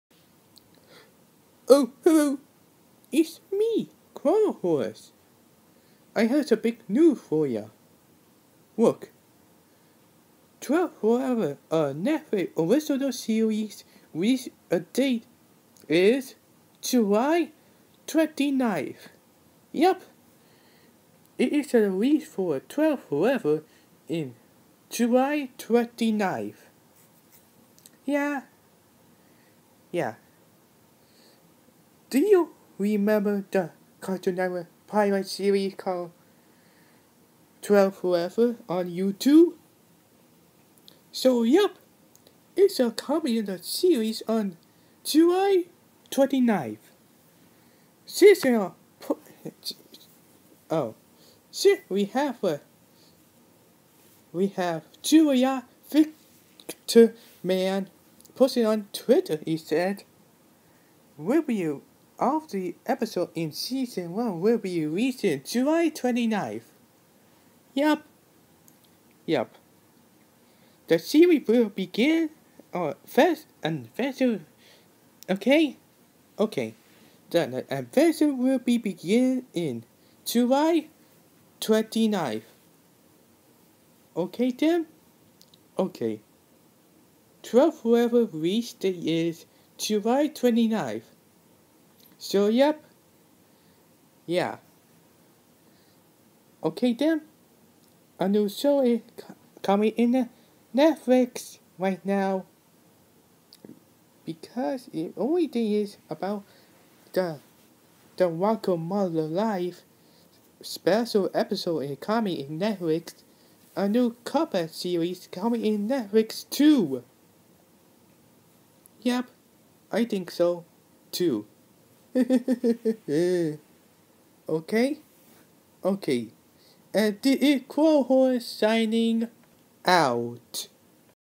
Twelve Forever Release Date is July 29th
I am excited about Twelve Forever series is coming in on Netflix on July 29th
Because remember the Cartoon Network pilot called Twelve Forever on You Tube well ok
(More Videos Stay Tooned)